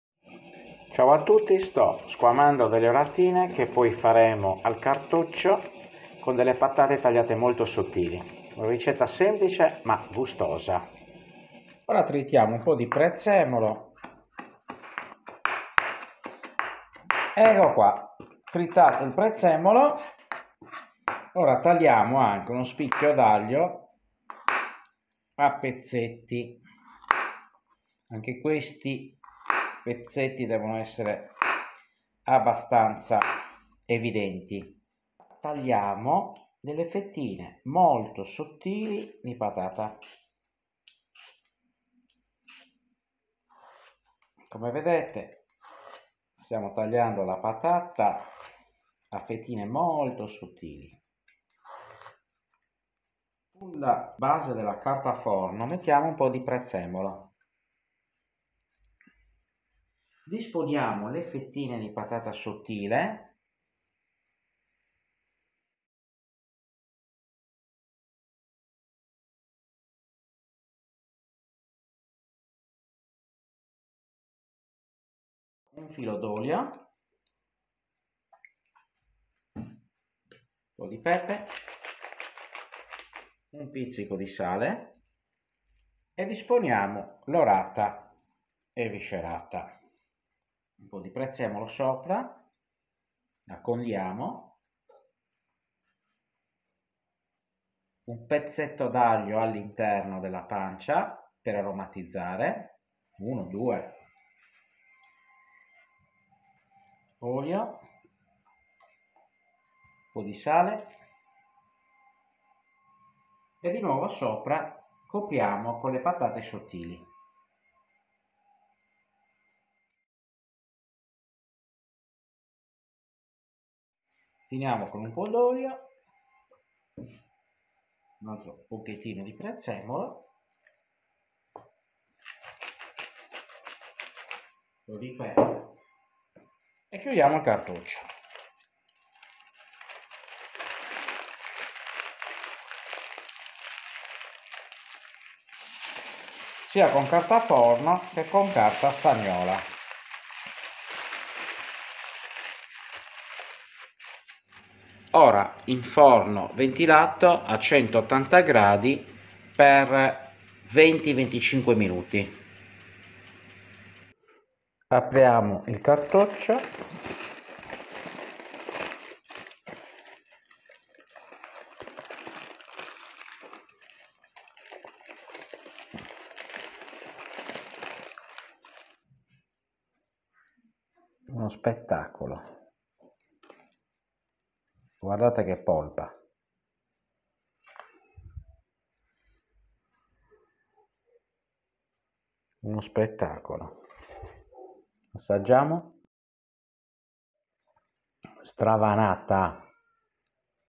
Orata al cartoccio con petali di patata
Orata al cartoccio con petali di patate

Oggi una cottura al cartoccio 😋

Ingredienti per 4 orate
1patata grossa
1spicchio d'aglio
1 ciuffetto di prezzemolo
Olio extravergine

Preparazione
- Squamare ed eviscerare l'orata
- Tritare il prezzemolo
- Sbucciare e tagliare a pezzetti l'aglio
- Pelare le patate e ricavare delle fettine trasparenti 😁

Procedimento 👨‍🍳
In un foglio di carta forno fai un lettino con il prezzemolo, le fettine sottilissime di patata, olio, sale e pepe.
Poi adagia l'orata pulita. Metti un pezzetto d'aglio nella pancia e condiscile come sotto. Olio, sale e pepe. Il prezzemolo e le fettine di patata a coprire tutto il pesce.
Chiudi il cartoccio e metti in forno per 25/30 minuti a 180°
Il tempo dipende dalla grandezza del pesce 😉
Io ho usato orate da 250 gr.

e premi il campanello per visionare le nuove ricette.
Grazie davvero per il tempo dedicato alla visione!

Ciaooo alla prossima ricetta 😁😁😁

Ciaoooo 🤗🤗🤗
Alla prossima ricetta 👨‍🍳